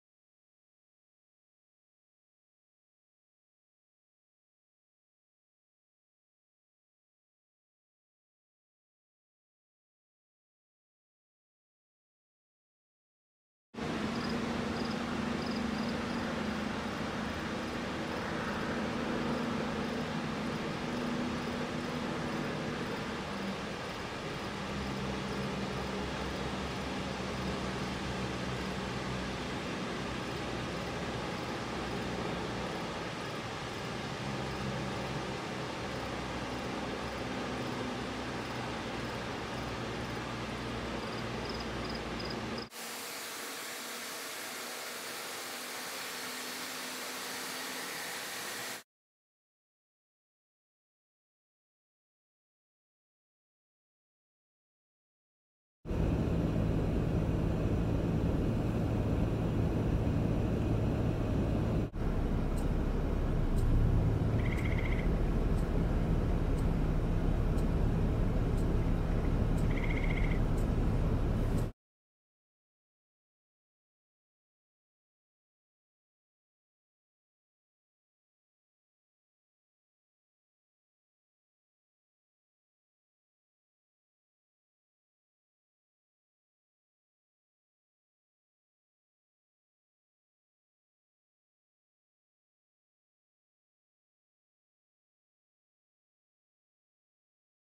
Night Drive
testing out the camera and getting ready to go to Minneapolis in a few days to tape at night there.